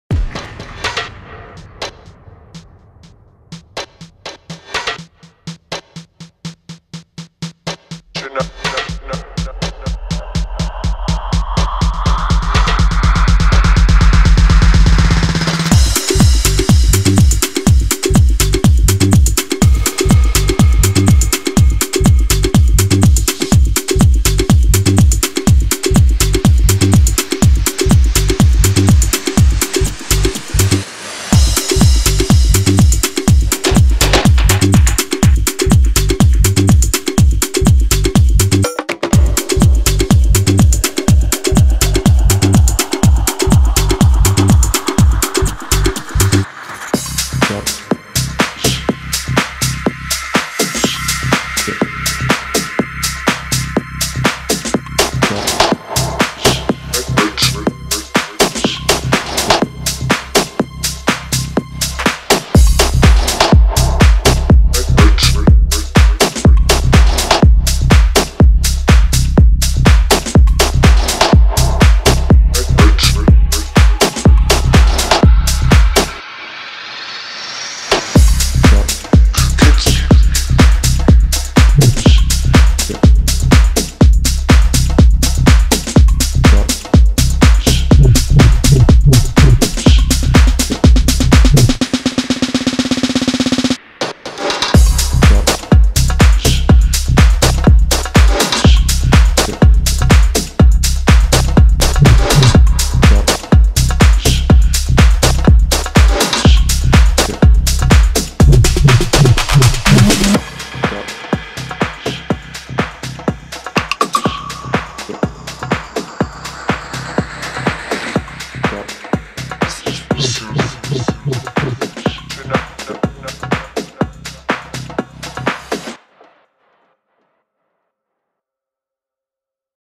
TECH SFX | Royalty Free FX Loops, Vocal Cuts, Snare Buildups, Impacts & More
"Tech SFX" from SHARP (Function Loops Label Group) delivers new set of FX sounds, but not just simple risers and boring splashes.

Expect to find those little "filler" sounds, such us interesting vocal bits or effected percussion. Of course, we added also the risers and splashes, but not much :)